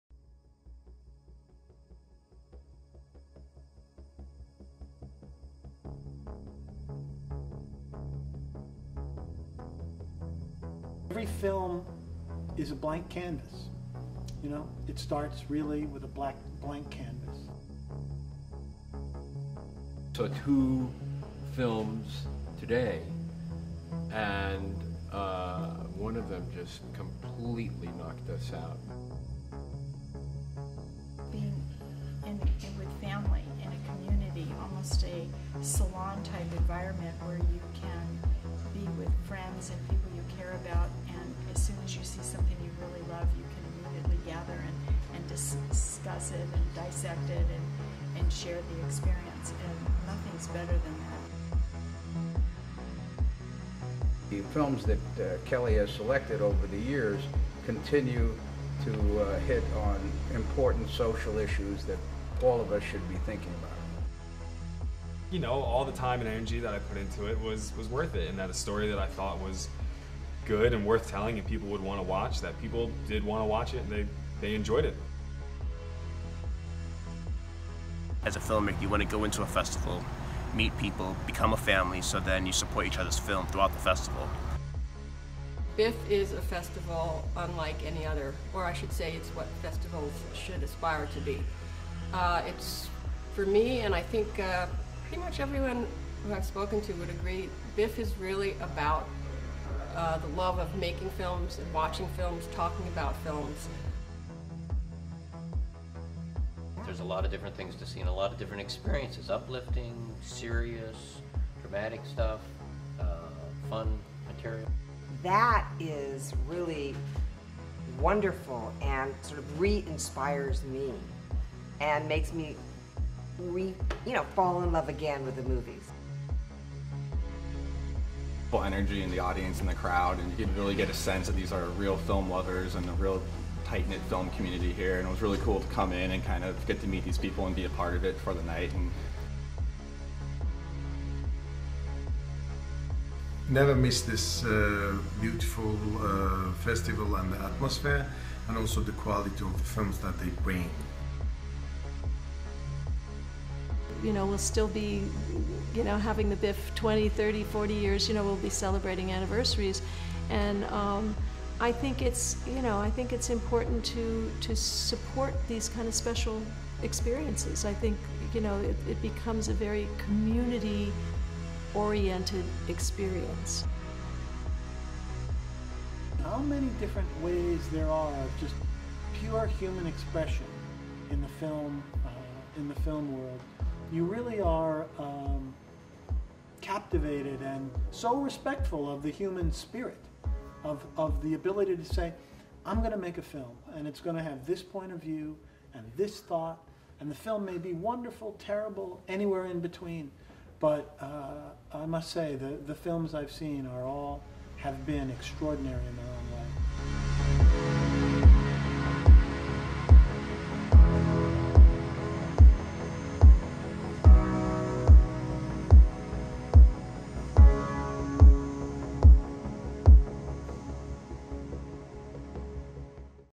The Berkshire International Film Festival - Be Extraordinary.
Hear from filmmakers, filmlovers, supporters, actors - about what makes the Berkshire International Film Festival a must-see event.